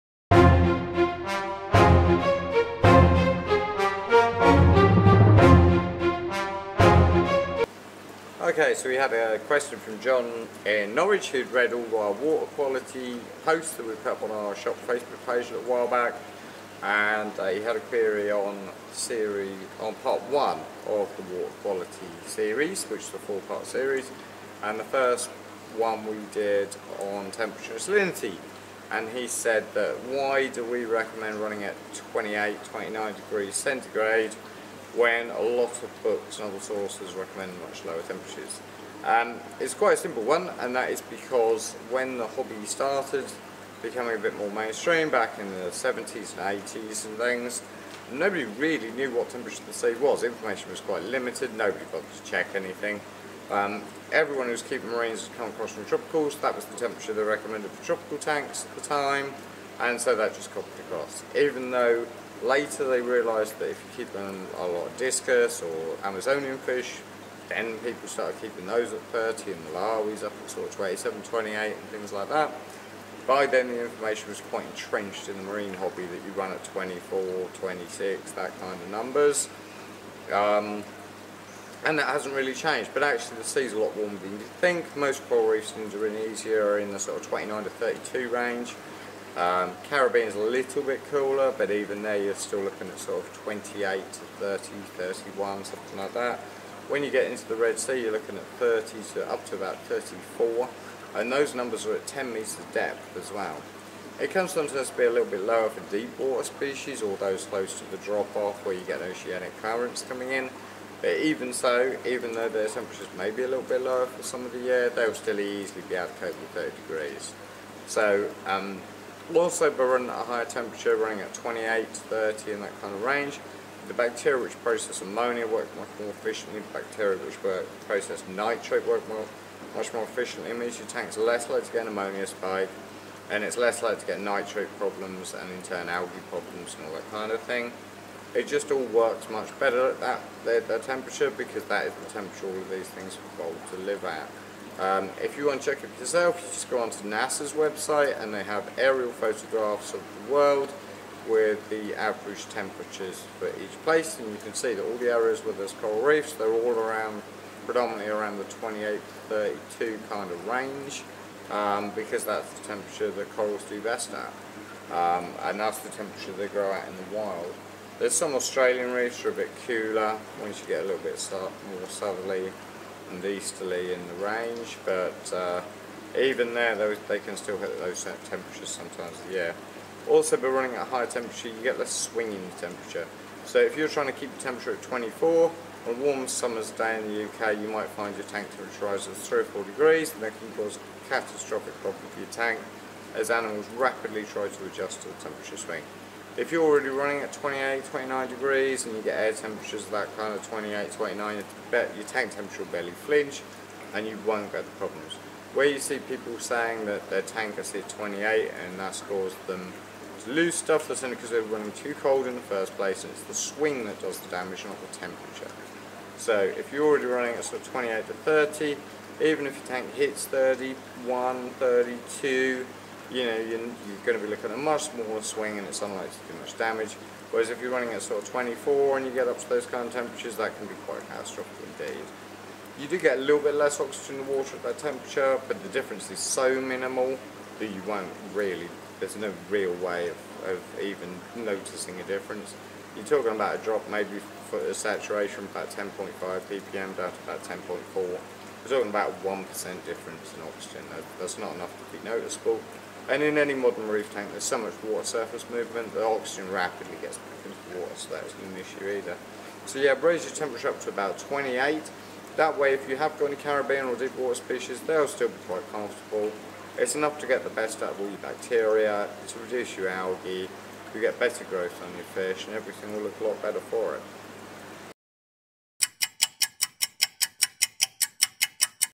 What TEMPERATURE Should My Reef Aquarium Be? Ask James
What temperature should my reef aquarium be?

Ask James, tackles a frequently asked question, what temperature should you run your marine tank. James simply explains what temperature you should run and and a brief explantation why.